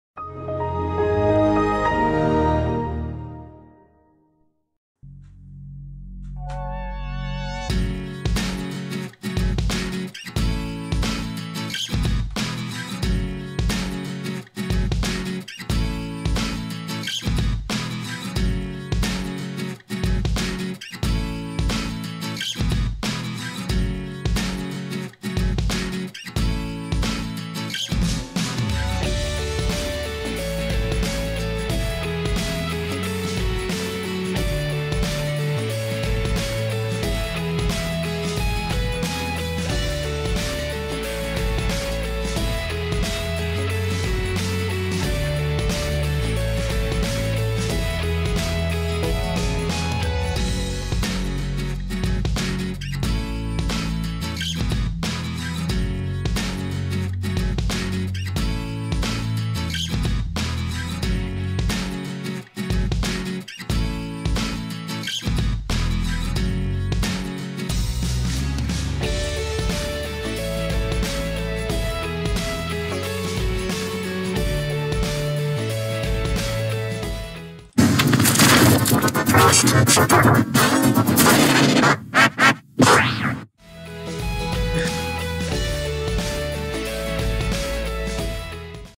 How To Make V-Major 16 On Sony Vegas Pro
Made Yesterday on ‎February ‎8, ‎2019, ‏‎1:42:24 pm to 1:43:41 pm

Let's make a V-Major 16 effect (G-Major 16 + V-Major) made without combining effects.

Note: If I did it wrong, just please give me the instructions, and I'll understand it.